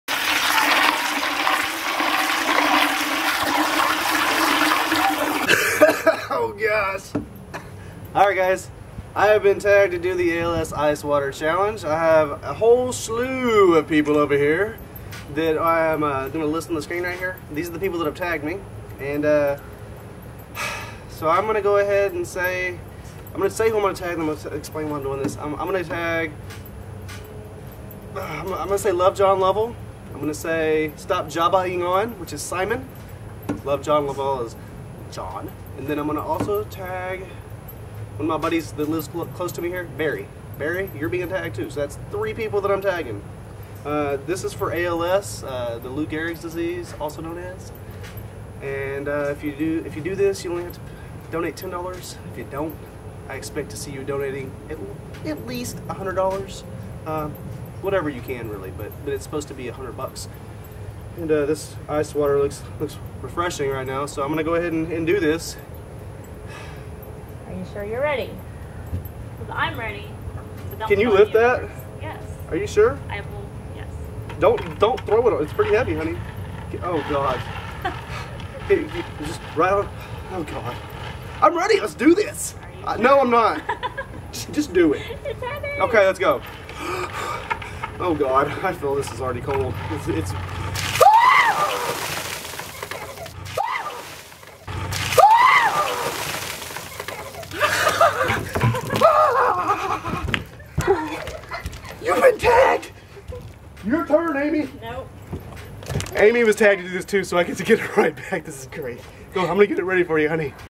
ALS Ice Bucket Challenge
I have challenged:
Jon "LoveJonLovell"
Simon "StopJabbaingOn"
Barry D.

Hope you enjoyed and please help if you can!